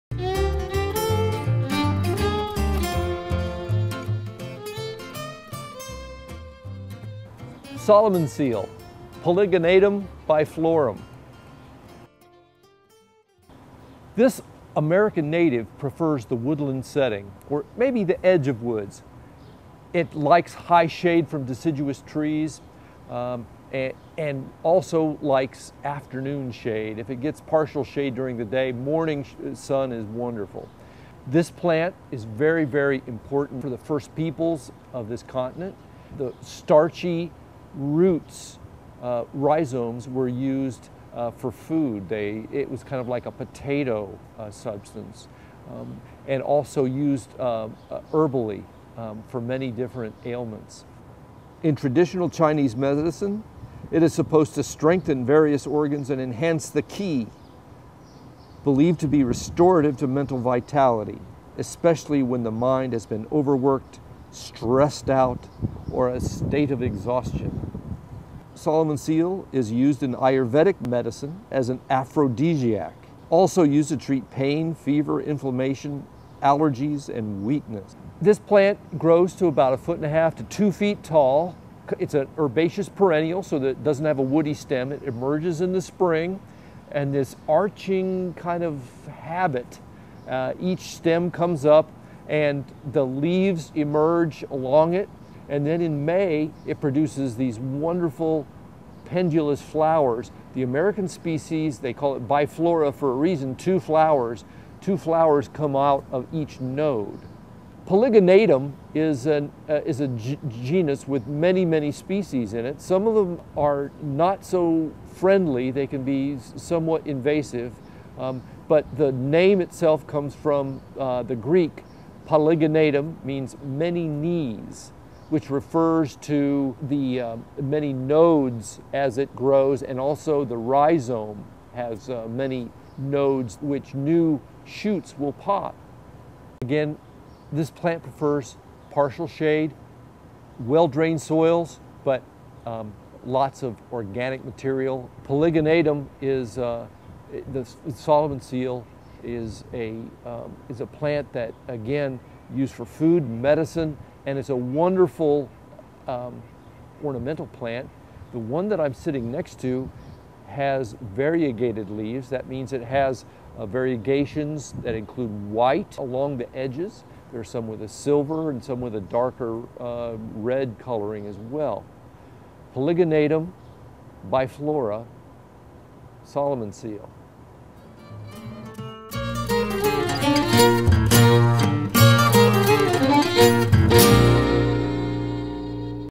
Solomon's Seal - Polygonatum biflorum - Ayervedic Aphrodesiac
Solomon's Seal - Polygonatum biflorum  is a classic shade garden plant that adds depth to garden beds, thanks to its arching stems. Super for woodland gardens.

In traditional medicine Polygonatum has been used in the treatment of diabetes and was first observed in 1930 by Hedwig Langecker who concluded that it was likely effective in fighting nutritional hyperglycemia.

Polygonatum is used in Ayurvedic medicine as an aphrodisiac and also to treat pain, fever, inflammation, allergy, and weakness.

Traditional Chinese medicine uses Polygonatum to strengthen various organs and enhance the qi. Polygonatum is believed to be restorative to mental vitality, especially when the mind has been overworked, overstressed, or is in a state of exhaustion.